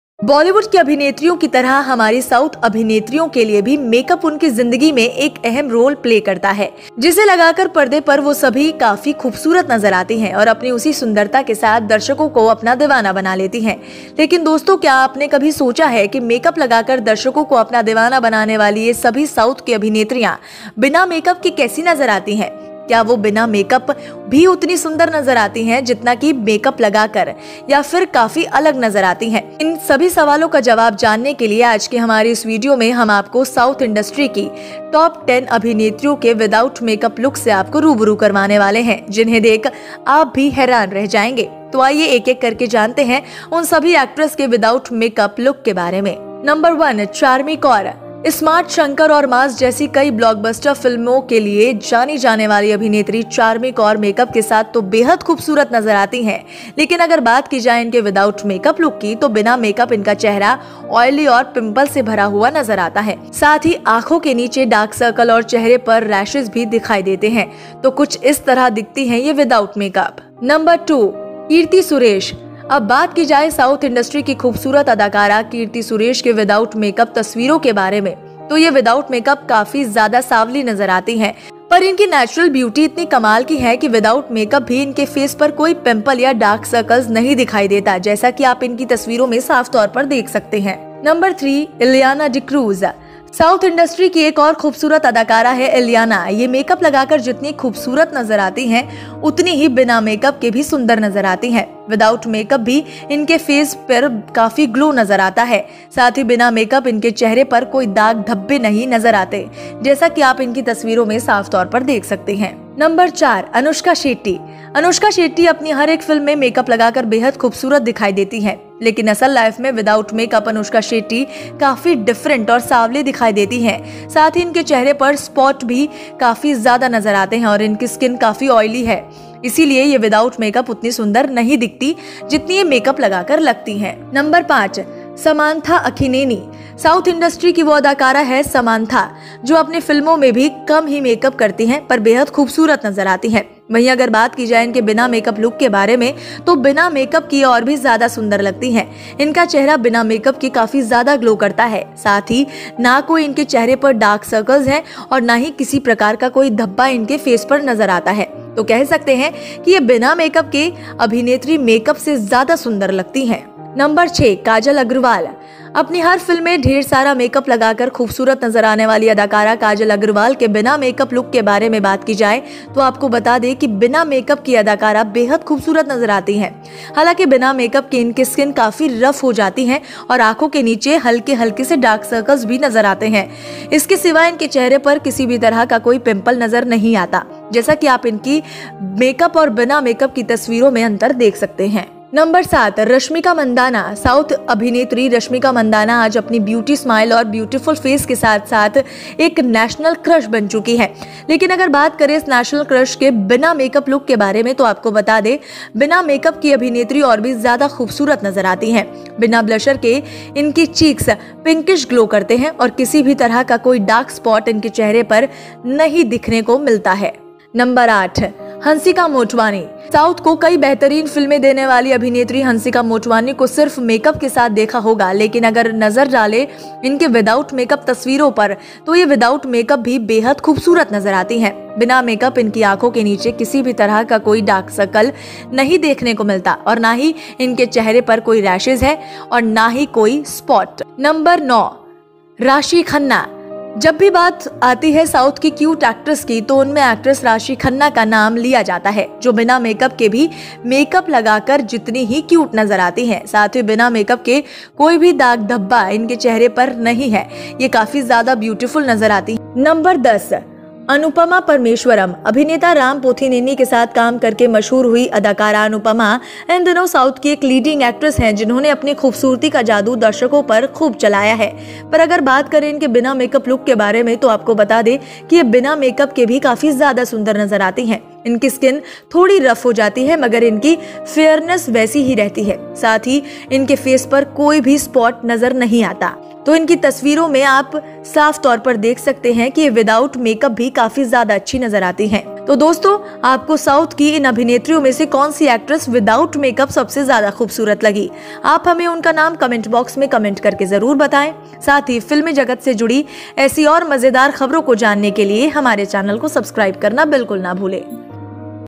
साउथ की ये सभी अभिनेत्रियां खूबसूरत दिखने के लिए मेकअप पर करती हैं लाखों रुपए खर्च South actress
Tital-top 10 Anupama Parmeshwaran without makeup Mahira

Like our Bollywood actresses, makeup also plays an important role in her life, which she looks very beautiful on screen and makes the audience crazy with her same beauty .

  But friends, have you ever thought that all the South actresses who make the audience crazy by applying makeup, how does it look without makeup, does she look as beautiful as without makeup or look very different.

  To know the answer to this question, in this video of our day, we are going to introduce you to the make-up look of 10 South Indian actresses, who will also be surprised to see you.

 So let's know one by one about all those actresses without makeup look: -

 No. 1 Charmi Kaur

 Known for many blockbuster films like "Ismart Shankar" and "Maas", actress "Charmi Kaur" looks gorgeous with makeup, but if she talks about her make-up look, then without makeup, her face is oily and pimple.  Looks full of light as well as dark circles under the eyes and rashes on the face, so this looks like this without makeup.

 Number 2 Kirti Suresh

 Now, to talk about the make-up pictures of the beautiful South Indian actress "Keerthy Suresh", this make-up looks very dark, but her natural beauty is so amazing that without makeup, we also have a pimples on her face or  Dark circles do not appear as you can see in these pictures.

 Number 3 Ileana Dikruz

 "Ileana Dikruz" is another beautiful actress from South Industry. She looks as beautiful with this makeup as it is beautiful without makeup, without makeup, her face beauty looks quite glowing as well without makeup.  There are no visible stains on the face as you can see in these pictures.

 Number 4 Anushka Shetty

 "Anushka Shetty" looks very beautiful by applying makeup in every one of her films, but in real life, without makeup Anushka Shetty looks quite different and dainty, as well as spots on her face and her skin is quite oily  This is why this makeup makeup does not look as beautiful as makeup does.

 Number 5 Samantha Akkineni

 The actress from South Industry is "Samantha Akkineni" who does very little makeup in her films but looks very beautiful, while if we talk about her makeup look then without makeup it would look even more beautiful.  If her face glows very much without makeup and neither does any postal appearance appear, nor does any type of blemish appear on her face, then it can be said that without makeup, this actress looks more beautiful than makeup  She comes.

 Number 6 Kajal Aggarwal

 Talking about the makeup look without the beautiful actress "Kajal Aggarwal", who has put a lot of makeup in each of her films, then tell you that without makeup, this actress looks very beautiful, although without makeup, her skin is quite  It gets rough and dark circles appear lightly under the eyes except that there is no pimple on their face as you can see the difference between their makeup and the pictures without makeup.

 Number 7 Rashmika Mandana

 South actress "Rashmika Mandandana" has become a national crush today with her cute smile and beautiful face, but if you talk about the makeup look without this national crush, then let us tell you that this actress without makeup is even more beautiful  Seen without blusher, their chicks are pinkish glow and somehow no dark spots are seen on their faces.

 Number 8 Hansika Motwani

 You might have seen "Hansika Motwani", the actress who gave many great films to South, but if you look at her without makeup photos, then this makeup makeup can also look very beautiful without any makeup under her eyes.  The dark circles are not seen nor any rashes are visible on their faces.

 Number 9 zodiac food

 Whenever it comes to the South's cute actress, she is named after her actress "Rashi Khanna", which looks as cute as she does without makeup, as well as being very beautiful without any stain stains without makeup.  Looks like you can see in these pictures too.

 Number 10 Anupama Parameswara

 Actress "Anupama Parmeshwaran", who worked with actor "Ram Pothineni", is a leading actress of South these days who has done her beauty magic on the audience, but if you talk about makeup look without them, then tell you  Even without makeup, it is quite beautiful, but their skin gets a little rough, but their fairness remains the same, as well as no spots are visible on their face, so it is something without makeup.

 So friends, which actress is the most beautiful without any of these actresses of the South, you must tell us by commenting her name in the comment box, and also to know more interesting news related to the film world, you can visit our channel  Do not forget to subscribe.